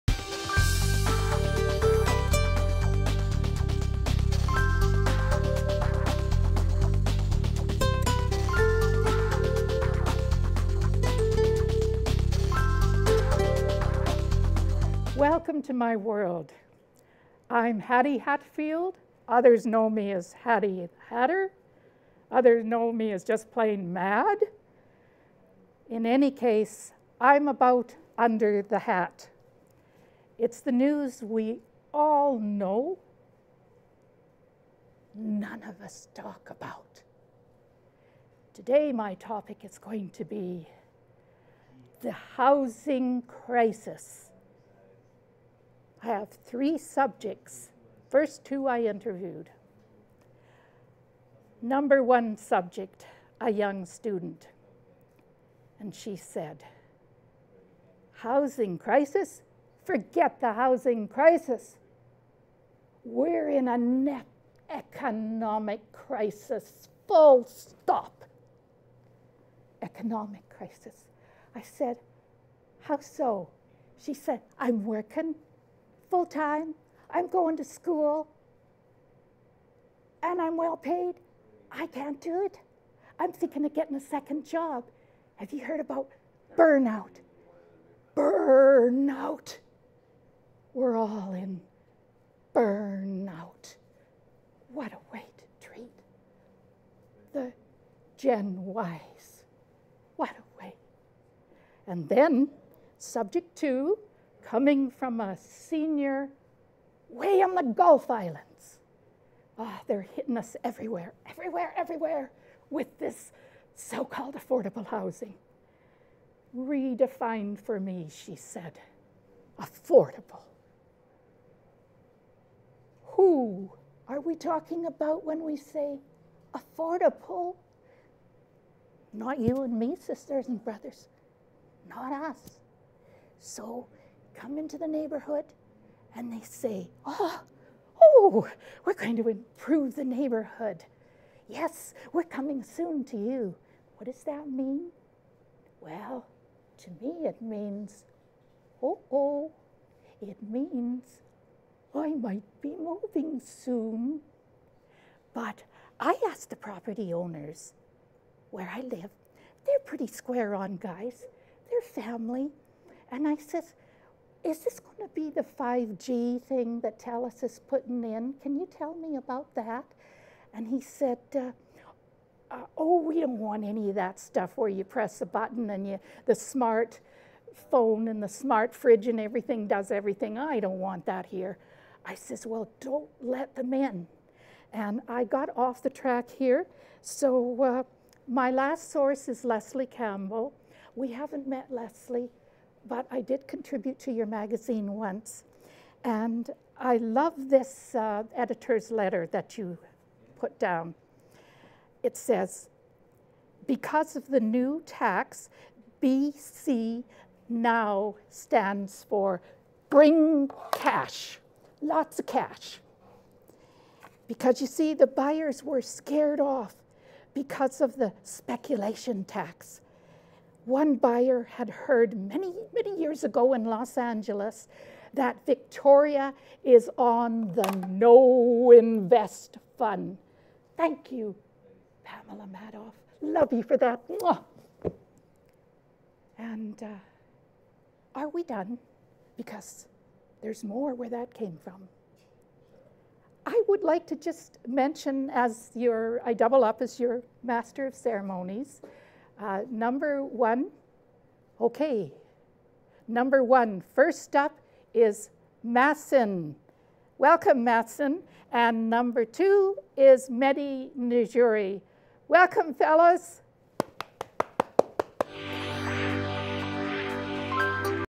Citizen's Forum 2019-02-27 #1: Hattie Hatter AKA Sheila on Housing & Living Wages
Citizen's Forum